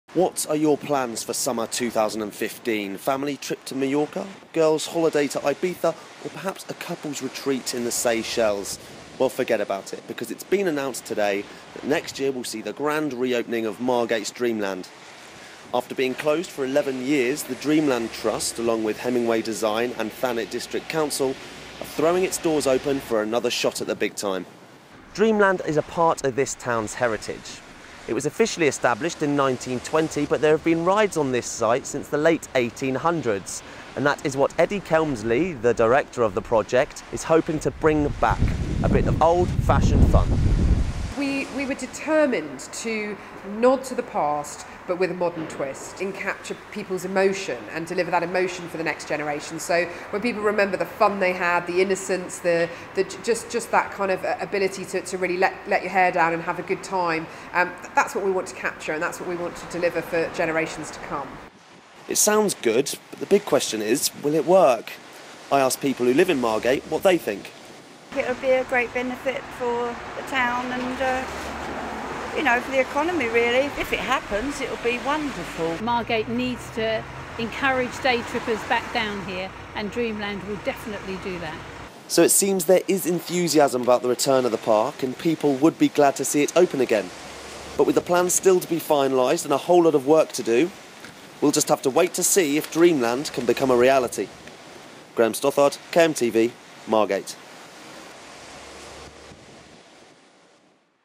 Dreamland
Dreamland Trust in Margate unveils plans for iconic amusement park.